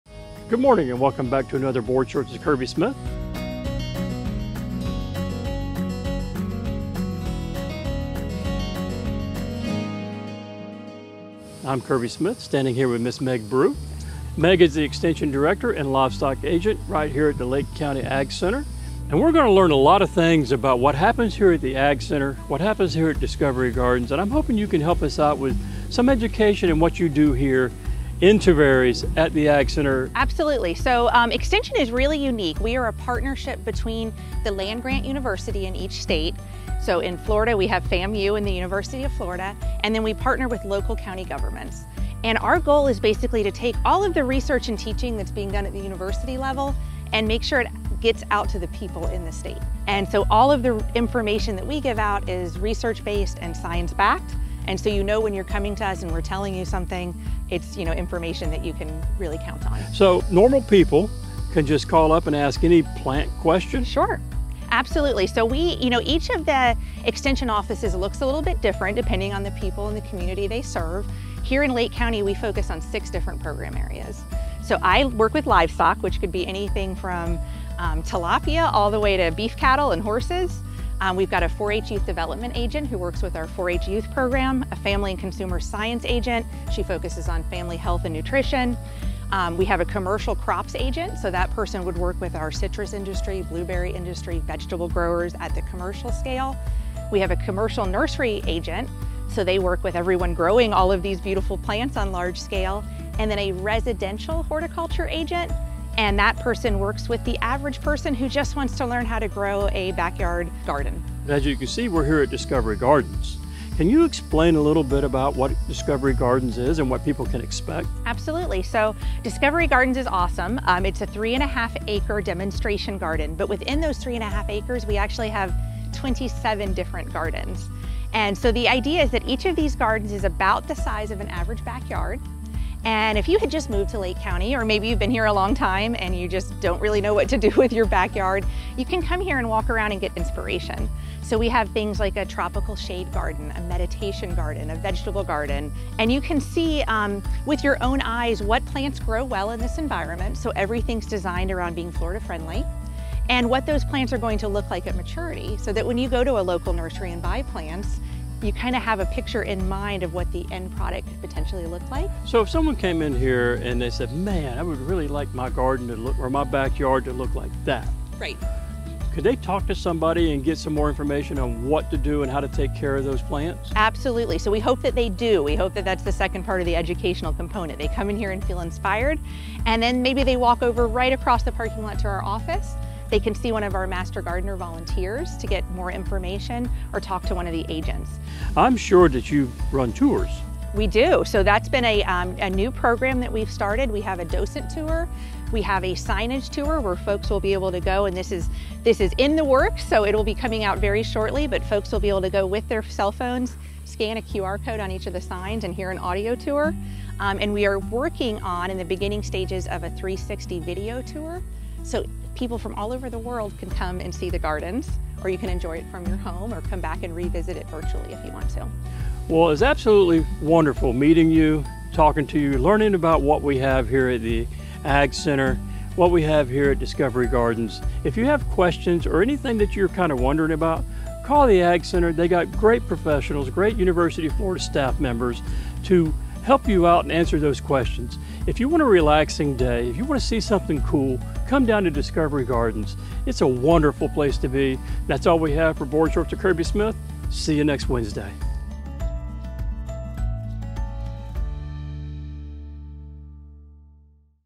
Board Shorts: Lake County's Agricultural Center
🌿A brand new episode of Board Shorts is out now! Join Chairman Kirby Smith as he explores the Agriculture Center in Tavares. Tune in now to learn more about the different programs and classes that their facility offers. Don’t miss out!